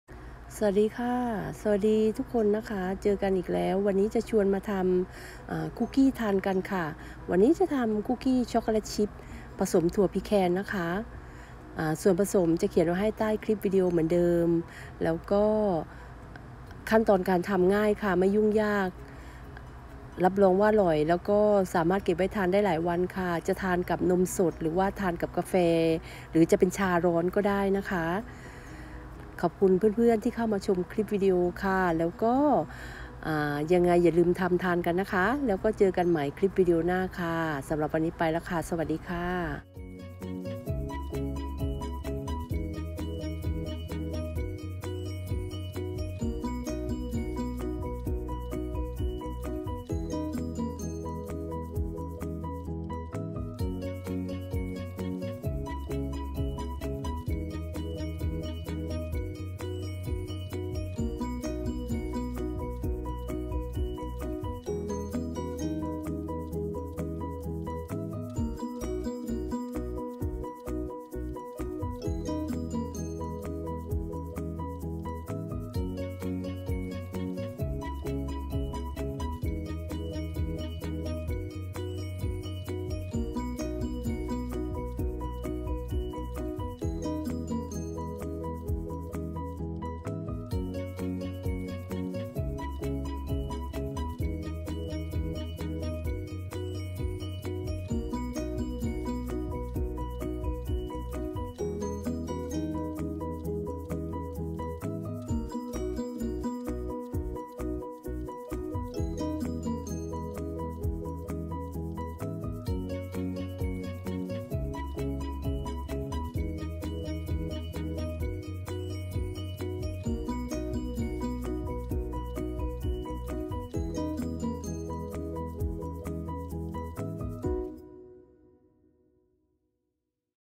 อบคุกกี้ทานกันค่ะ ช้อกโกแลตชิพคุกกี้ชีวิตในอเมริกา
Chocolate chips pecan cookies
แป้งเอนกประสงค์  2 1/2 ถ้วยตวง
น้ำตาลทรายขาว   1/2 ถ้วย
น้ำตาลทรายแดง   3/4 ถ้วยตวง
เกลือบ่น  1 ช.ช
Baking soda. 1 1/2 ช.ช
เนยสด   1/2 ถ้วย
กลิ่นวนิลา 1 ช.ช
ไข่ไก่ขนาดกลาง  3 ฟอง
อบ อุณหภูมิ350 F
เวลาอบ  12-14 นาที